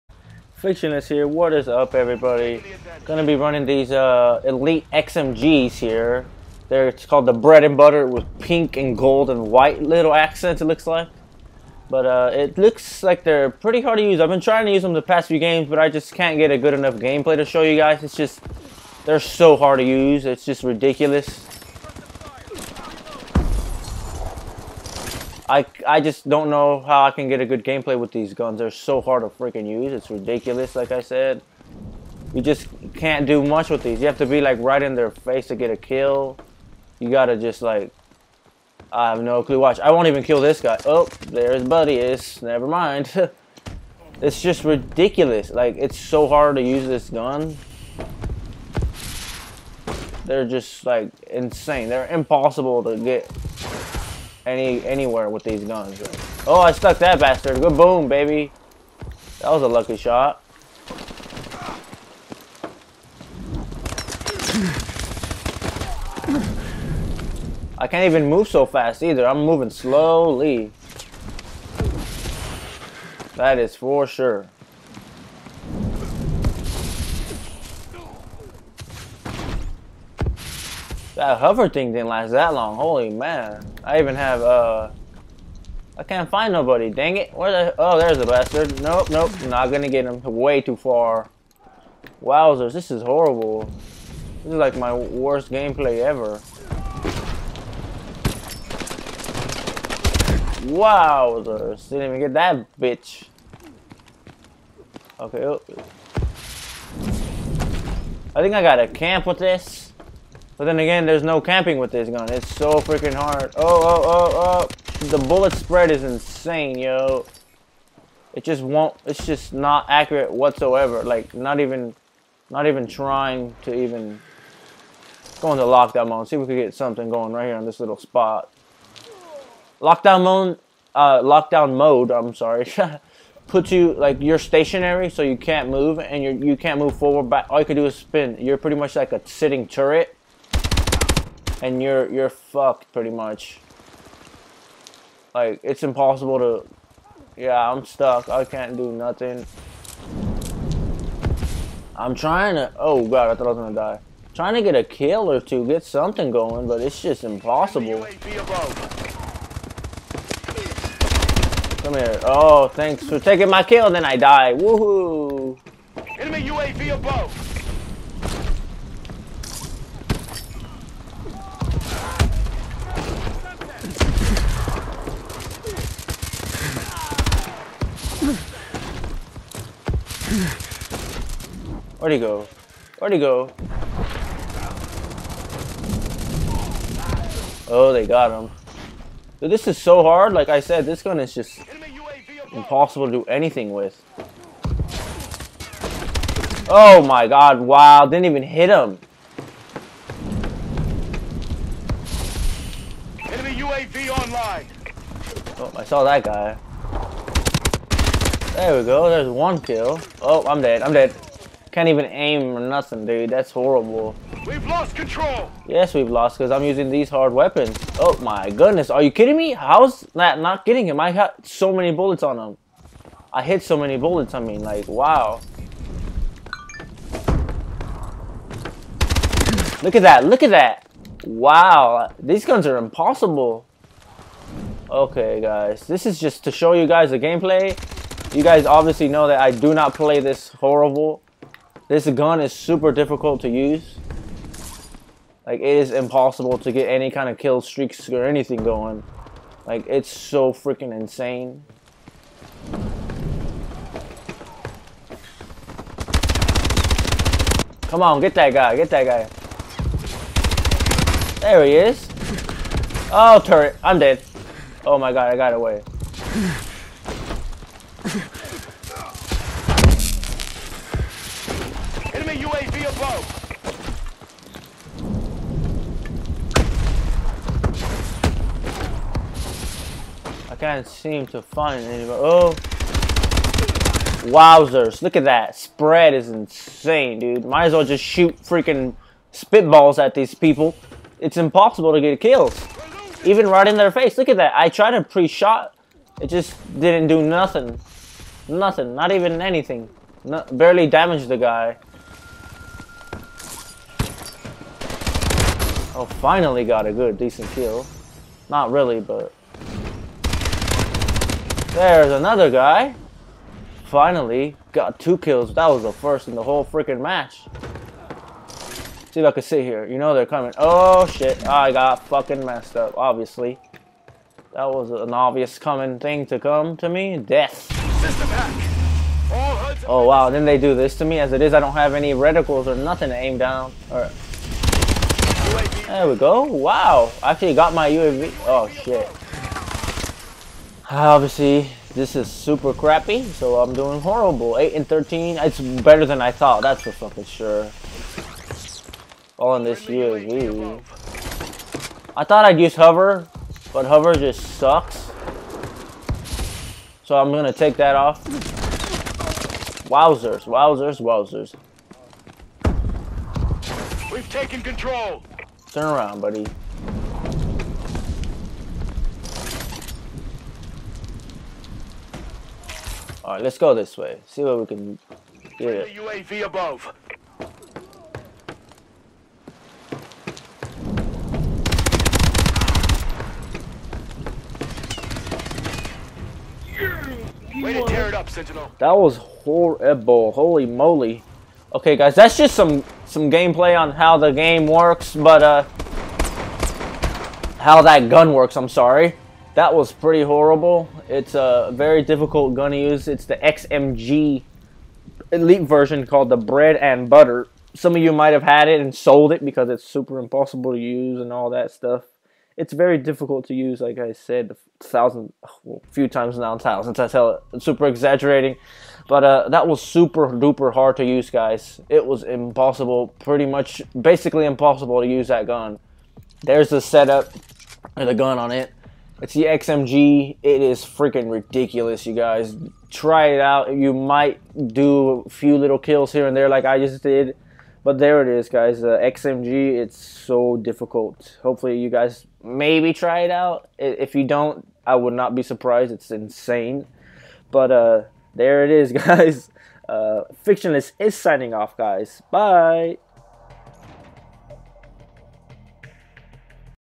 Heavy Weapon XMG Elite Version!
This Elite version isn’t ny better then the original version. It is near to impossible to get any killss with this gun!